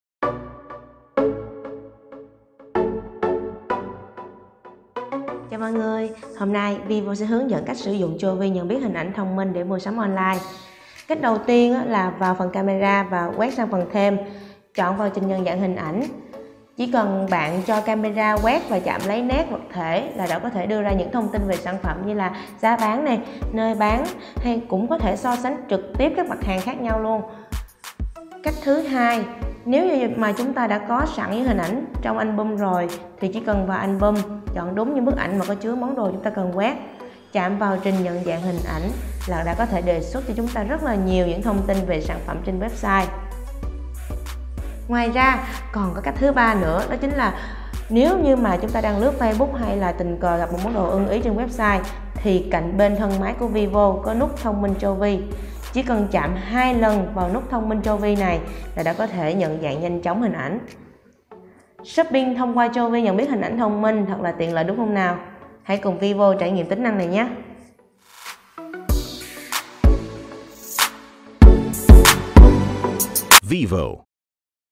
Mua hàng qua hình ảnh nhanh chóng trên điện thoại vivo
Nhu cầu mua sắm trên mạng với mọi người đã trở nên quen thuộc vì tính nhanh chóng và tiện ích của nó. Tuy nhiên, nếu bất ngờ bạn bắt gặp món đồ nào đó trên facebook hoặc báo mạng nào đó nhưng lại không biết thông tin về nó thì làm sao? Đừng lo, điện thoại vivo đã có tính năng mua hàng bằng hình ảnh sẵn sàng giúp bạn.

Cụ thể, với 3 cách của chế độ Jovi thông minh: tìm hình qua camera AI điện thoại, nhận diện hình ảnh trong album có sẵn và cuối cùng là tìm thông tin sản phẩm bất kỳ của hình ảnh trên mạng qua nút Jovi bên cạnh thân máy.

Những tính năng thông minh này thật sự tiện lợi đối với các cô nàng yêu thích shopping. Hãy cùng vivo trải nghiệm tính năng này nhé!